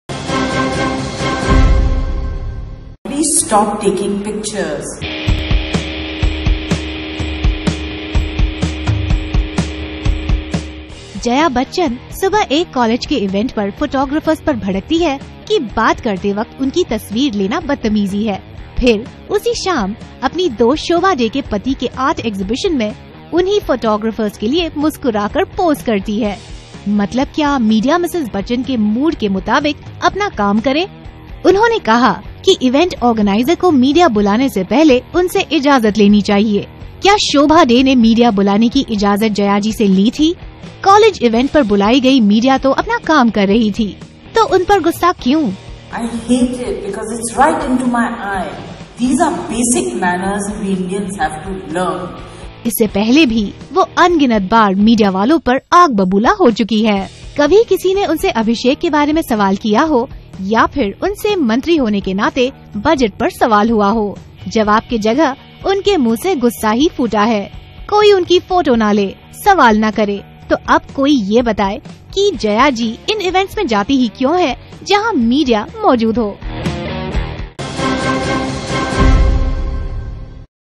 &lsquo;Aaj Ka Star Punch&rsquo;: Jaya Bachchan&rsquo;s rude reprimanding punch!
Jaya Bachchan slams media once again. Is she taking media for granted?